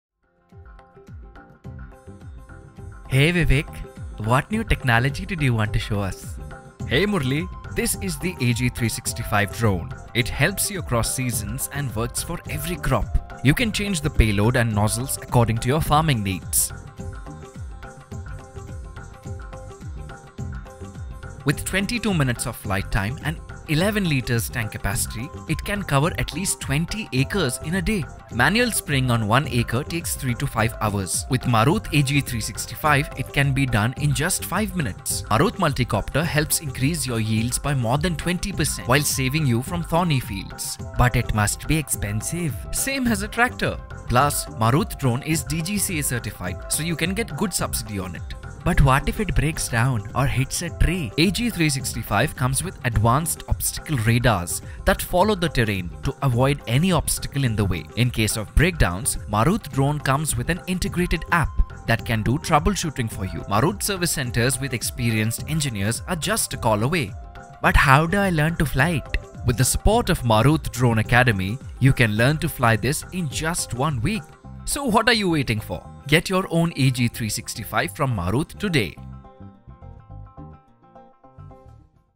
Marut Drones- Agricultural Drone Manufacturer in India | AG 365 | Kisan Drone | DGCA Certified
India's 1st Multi Utility Drone AG 365 is also DGCA Certified, so you can avail Loan, Subsidy, and Insurance with ease.

With AG 365 you can perform various Agricultural tasks like Spraying, Direct Seeding, Thermal Fogging, and Fertilizer Spreading etc.

Own AG 365 today.